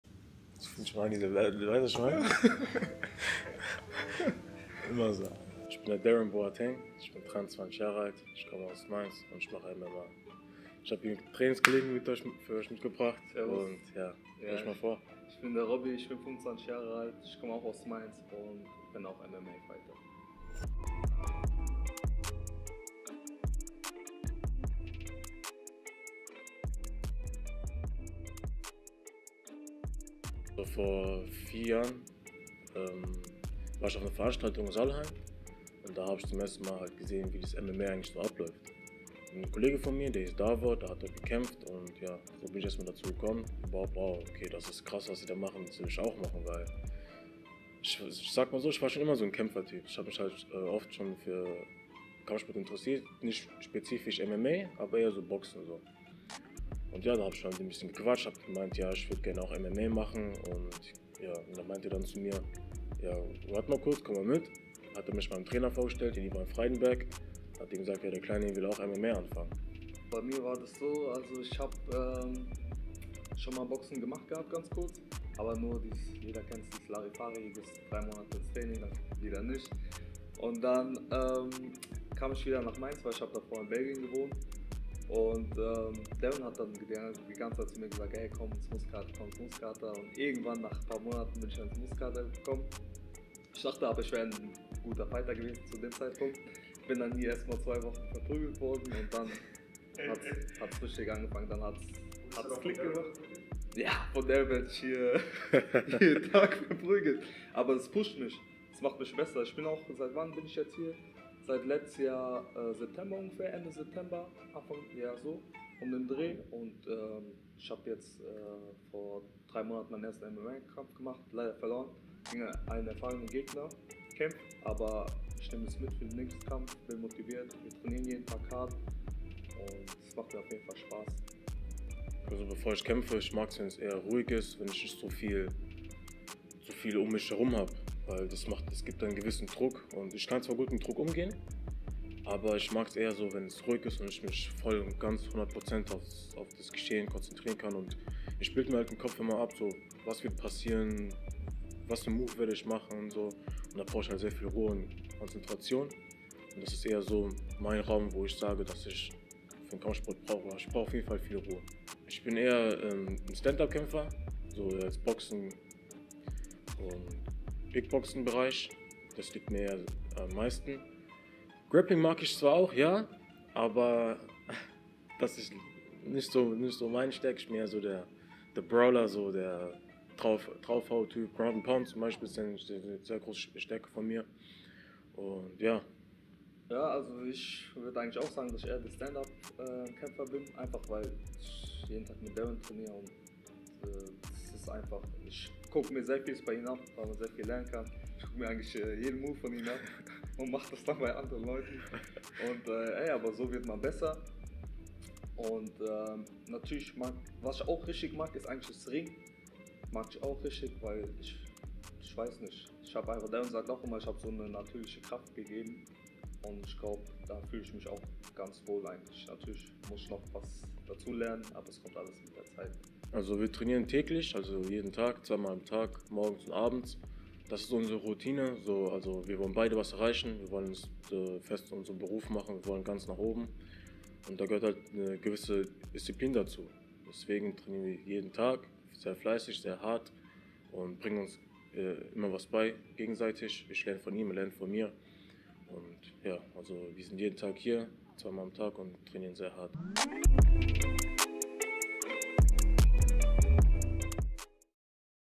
Deron & Robbie | MMA | Whats your Key?
In der 1. Folge unseres neuen Projekts "Whats your Key" stellen wir euch die beiden MMA-Kämpfer Deron Boateng und Robbie N´silu Ndongala! Die angehenden Profikämpfer trainieren im Kampfsport und Fitnessstudio Muskelkater in Mainz. Dadurch haben Sie die Möglichkeit sich gegenseitig zu motivieren, miteinander zu trainieren und zusammen zu wachsen! Trotzallem haben beide einen gewissen KEY, der sie ans Limit ihrer Fähigkeiten bringen kann.

Viel Spaß beim Interview! TRUST UR PROCESS